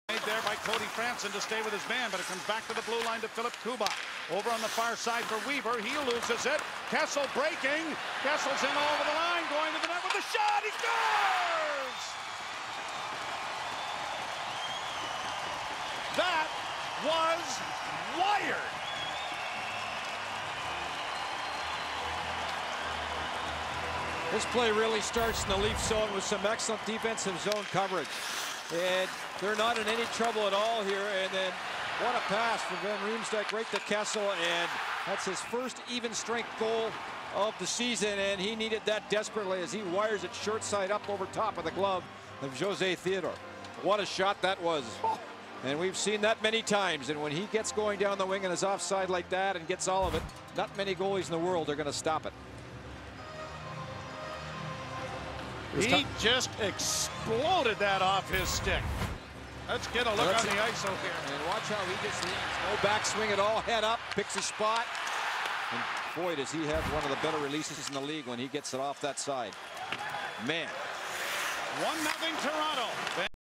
Kessel's Goal - Leafs 1 vs Panthers 0 - Feb 18th 2013 (HD)
What a Snipe!!!
1st Period - 17:04 - Phil Kessel (3) Snap Shot - Assists: J. van Riemsdyk (3)

Toronto Maple Leafs @ Florida Panthers
NHL 2013 Reg-Season